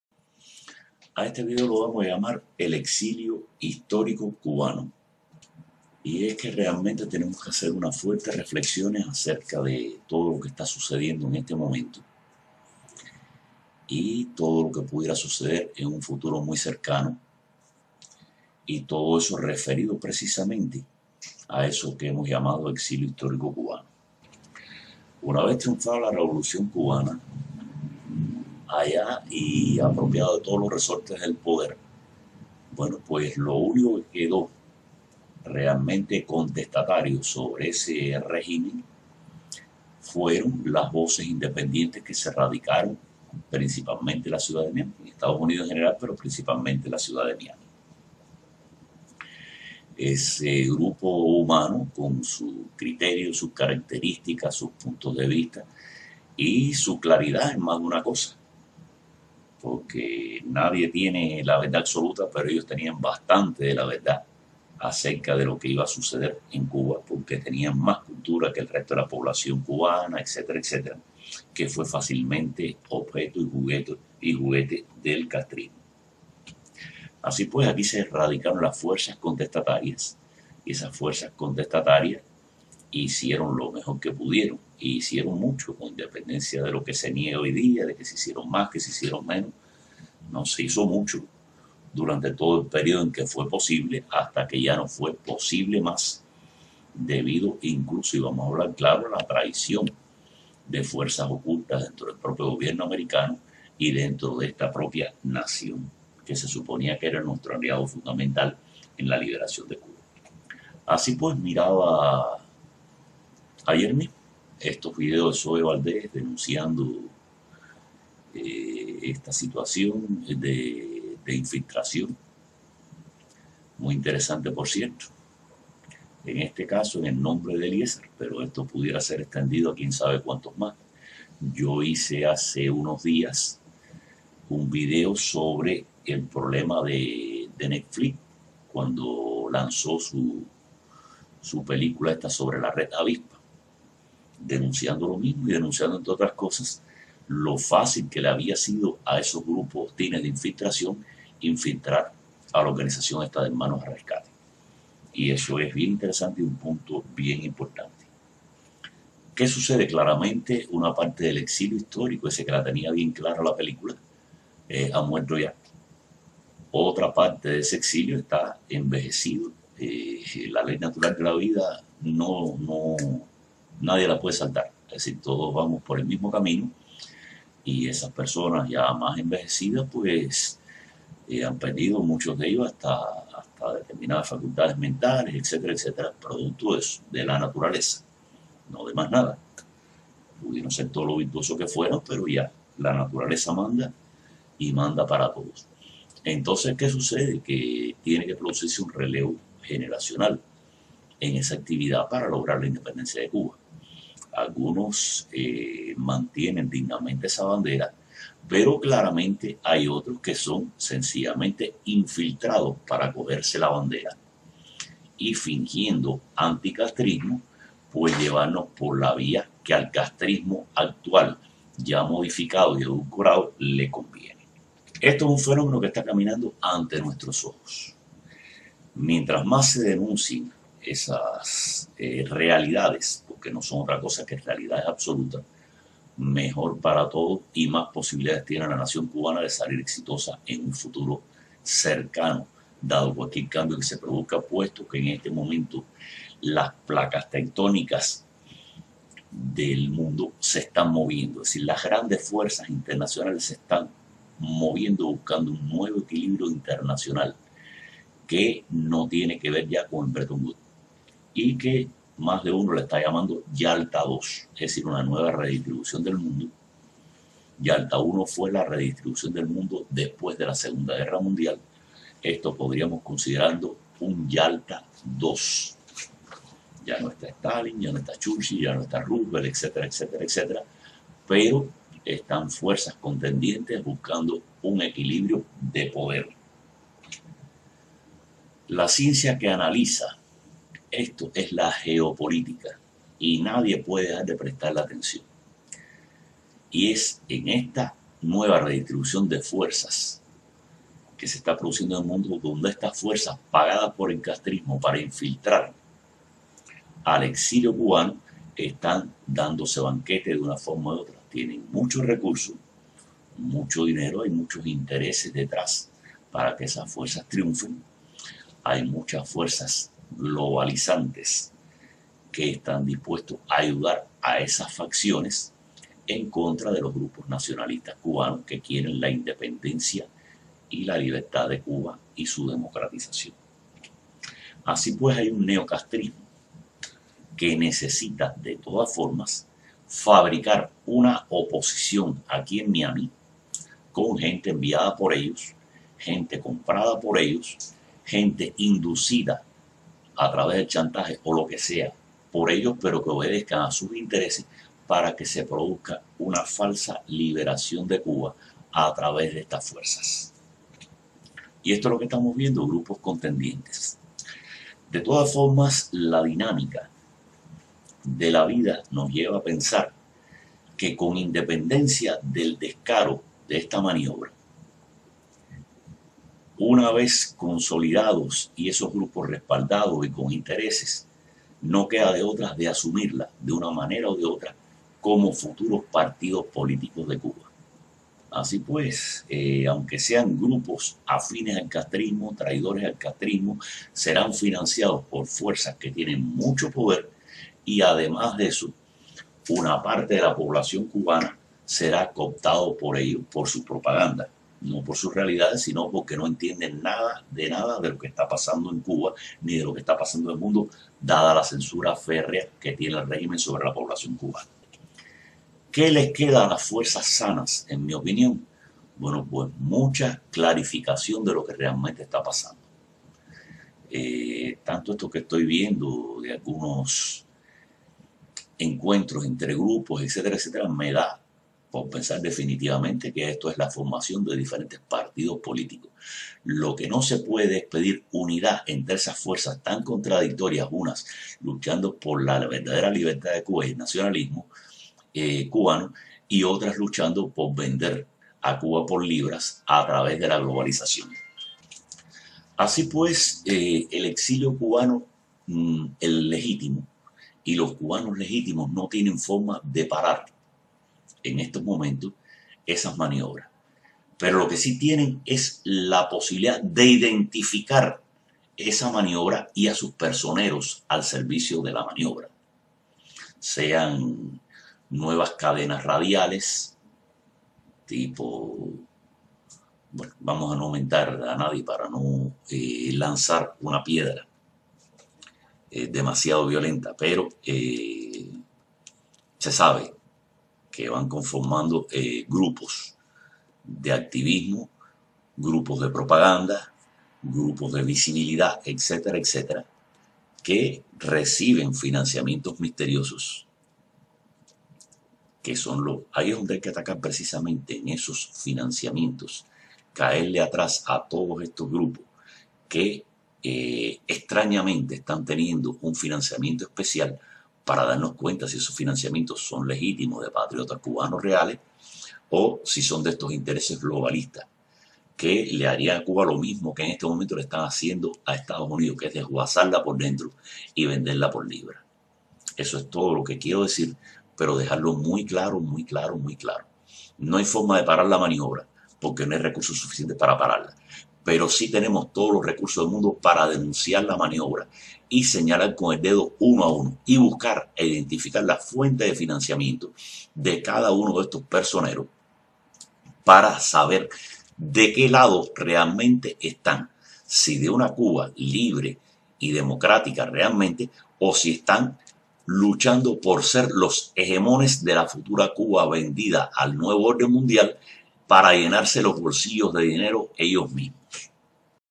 EL EXILIO HISTORICO CUBANO mpeg4
Un analisis acerca de la infiltracion del castrismo en Miami